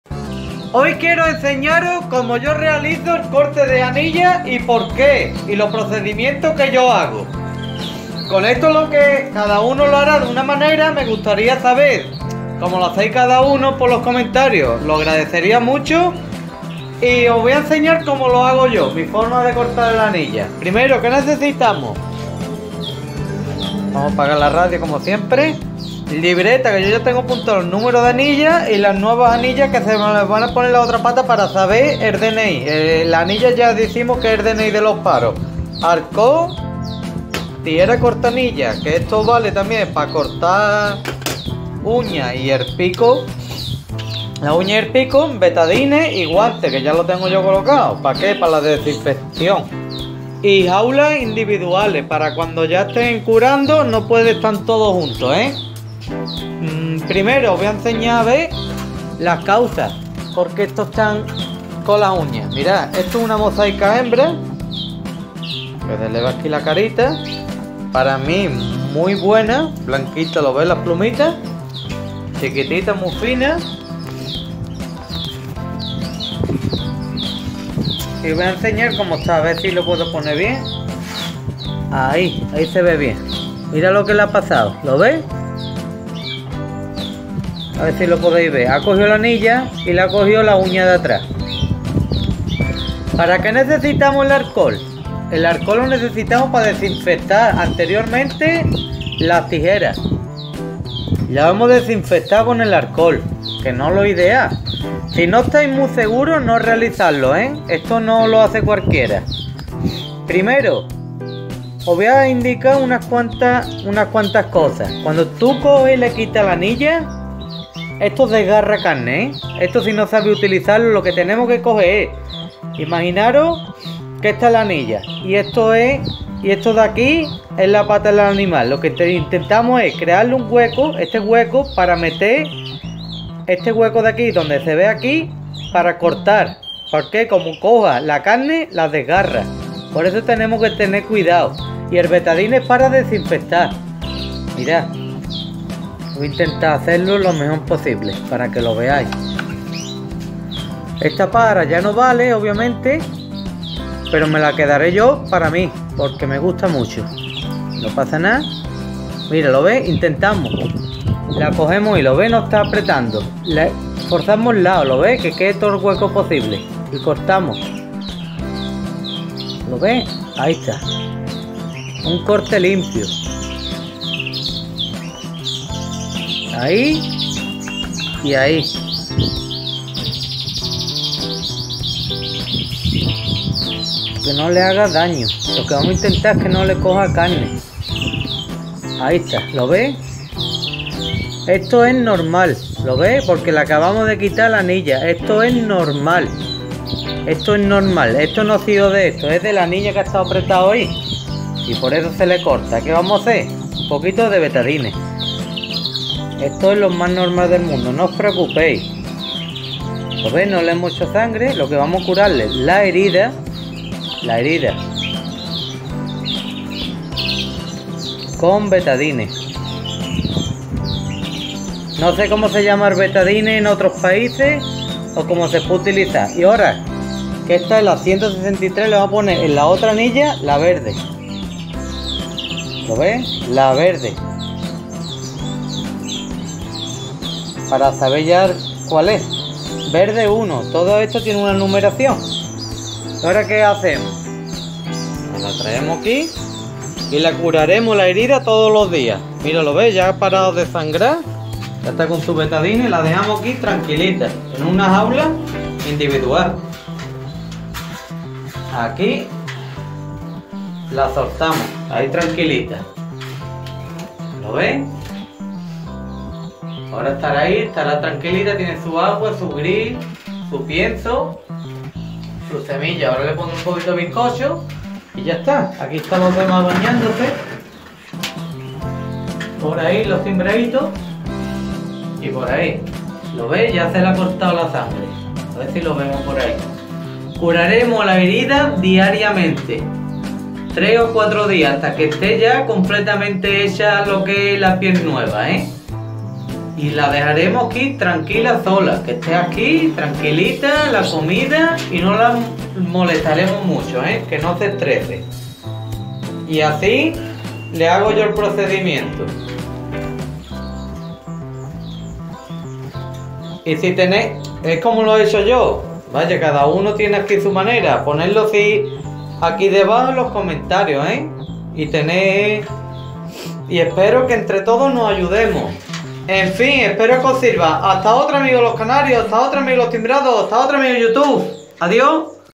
Cómo Sacar Un Anilla A Un Canario O A Cualquier Ave.
Juán Burgos nos enseña cómo sacar un anilla a un canario o a cualquier ave, cuando esta le aprieta la pata y se le empieza a inflamar.